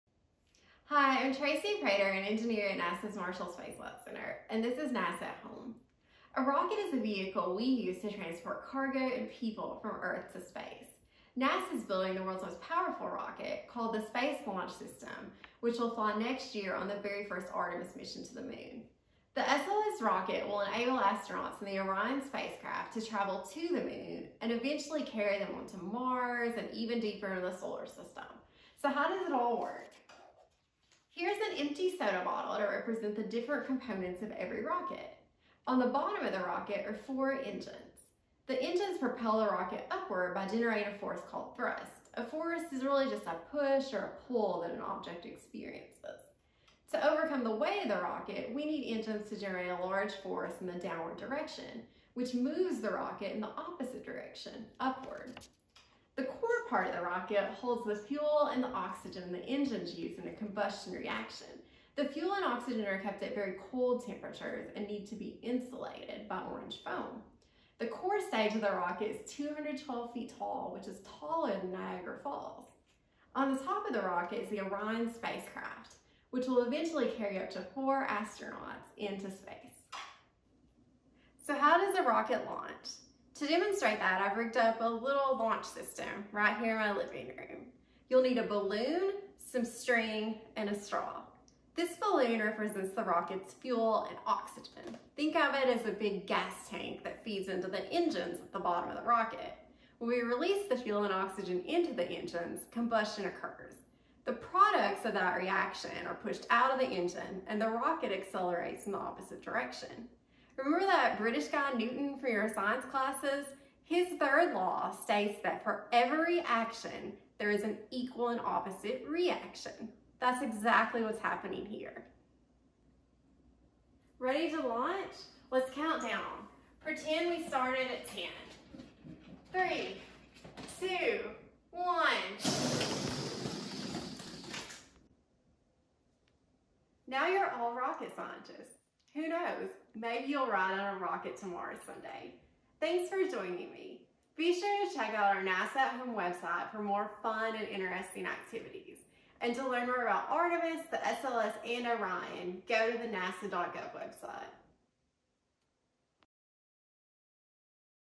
NASAatHome: How Does the SLS Rocket Fly
NASA’s powerful Space Launch System (SLS) rocket will launch NASA’s Artemis missions and its astronauts to the Moon. But how does a rocket fly?

In this NASAatHome video, Tracie Prater, an aerospace engineer at NASA’s Marshall Space Flight Center in Huntsville, Alabama, explains the basic components that help a rocket launch and fly beyond Earth’s orbit to deep space destinations like the Moon and Mars.

Students can follow along and make their own SLS rocket with these SLS-themed NASA at Home activities,
(NASA)